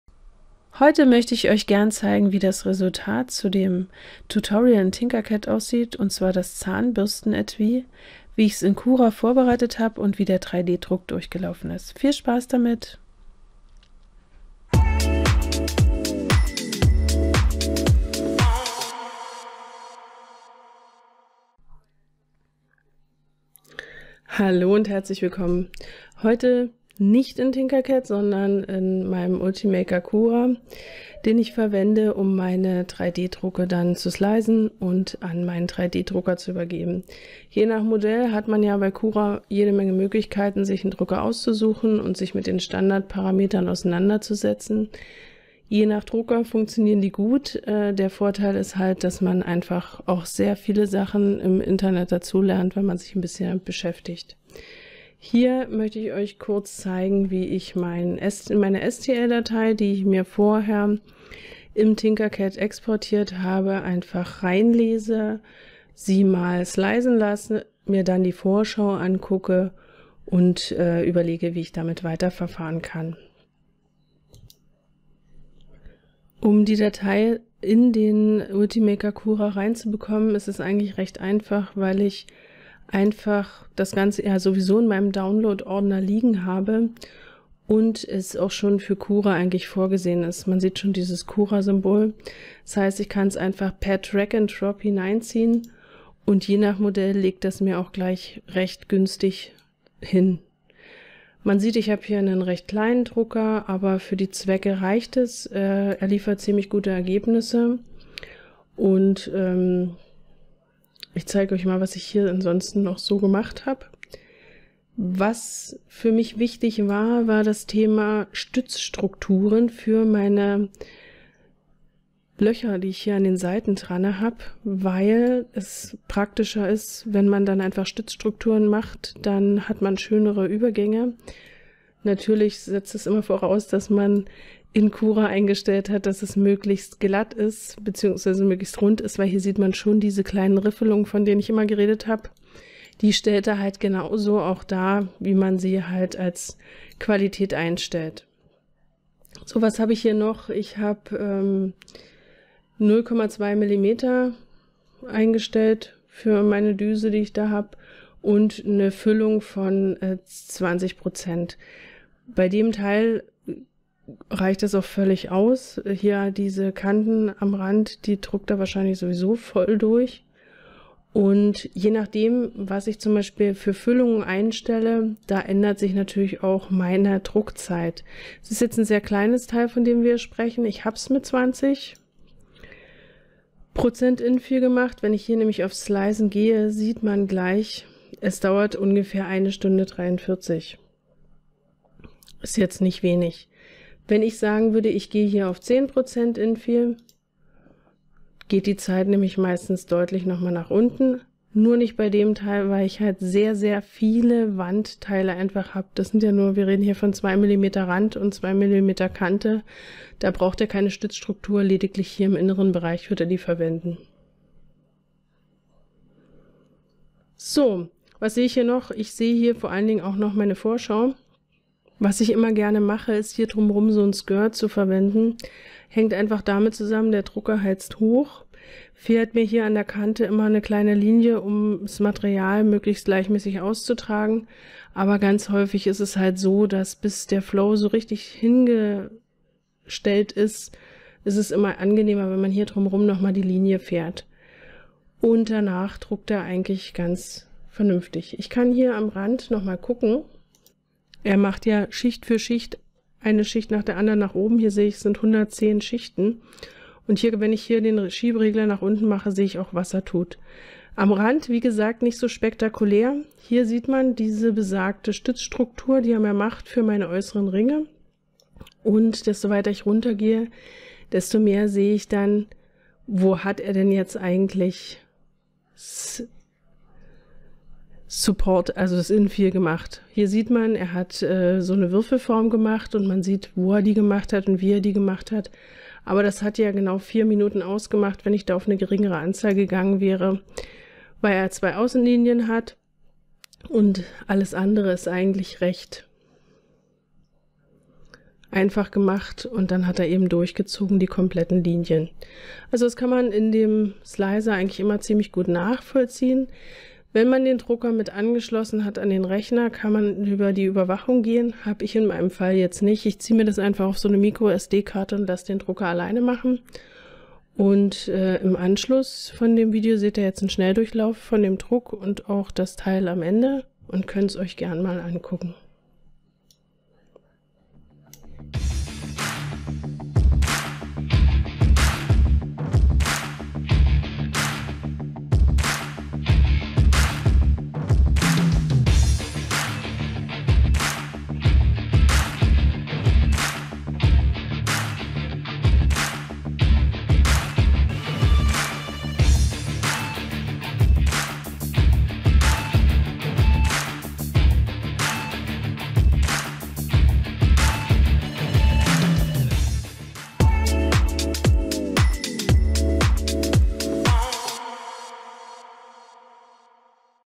3D Druck zum Zahnbürstenetui aus Tinkercad Tutorial
In diesem Video seht ihr wie der erste Prototyp zum Zahnbürstenetui gedruckt wird. Ich arbeite aktuell mit dem Slicer von Cura (Version 4.13) und habe einen kleinen Ender 2 Pro im Einsatz. Das Druckvolumen ist zwar nur 160 x160x180 mm aber für den Einstieg super. Außerdem stimmt Preis-Leistung hier sehr!

Ich habe hier nur ganz kurz die Oberfläche von Cura angerissen, kann aber gern mal ins Detail gehen wenn ihr mögt.

Kapitel
00:00 Einführung
01:10 STL bei Cura einspielen per Drag & Drop
02:00 Stützstruktur ja oder nein?
02:40 Infill anpassen
03:40 Vorschau und  wozu eigentlich ein Skirt verwenden?
05:20 Schnelldurchlauf des Druckers